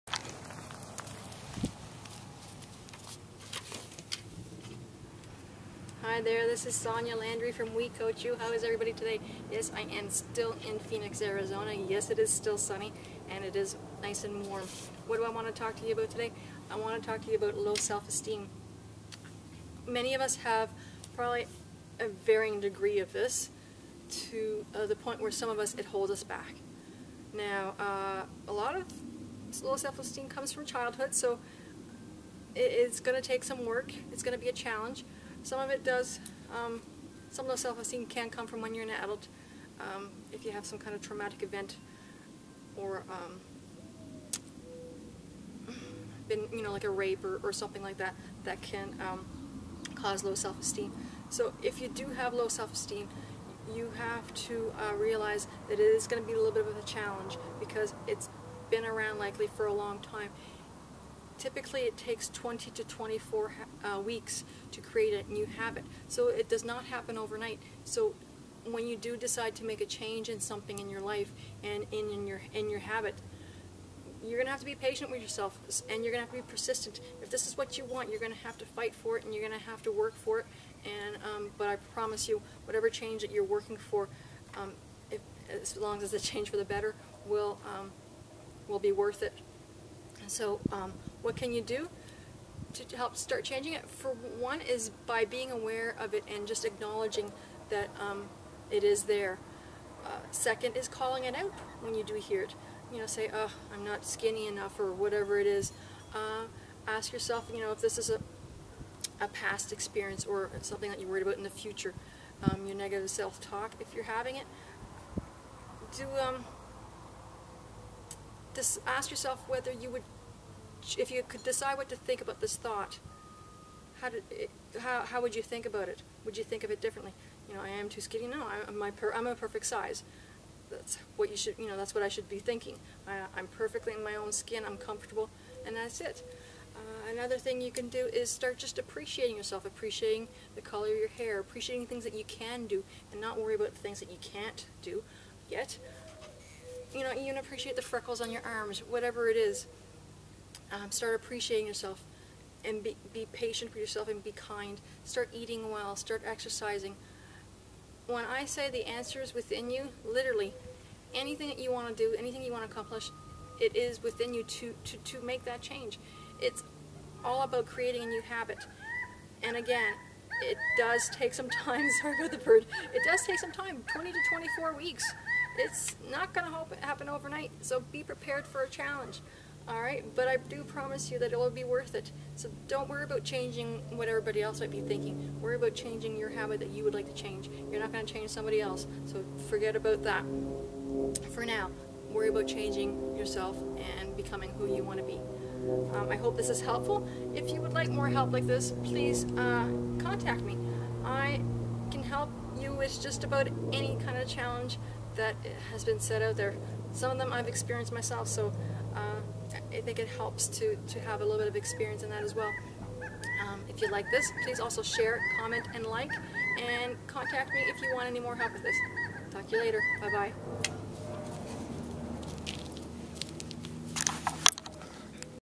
Yes You Are, Wonderful! Sonia Landry WeCoachU
Yes You Are, Wonderful!  Sonia Landry from WeCoachU teaches you how to improve your self esteem and become anything you want.